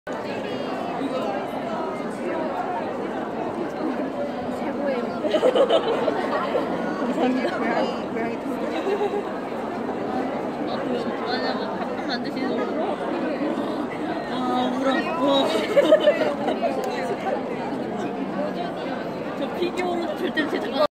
튜닝타임즈 프라모델 강좌 용산팀 OOO님의 Nendoroid Custom 넨도로이드 커스텀 Overwatch Tracer 오버워치 트레이서
용산 아이파크 문화센터 넨도로이드 커스텀 수업 수강생 OOO님이 7월 첫째주 프로젝트돌 행사장에서 직접 바느질로 마감해준 의상을 입힌 오버워치 트레이서를 세팅해주셨는데요, 남자아이처럼 반바지를 입혀서 더욱 보이쉬한 매력을 부각시키면서 귀여운 모습으로 마무리를 지어 주셨습니다^^ 프라모델 튜닝타임즈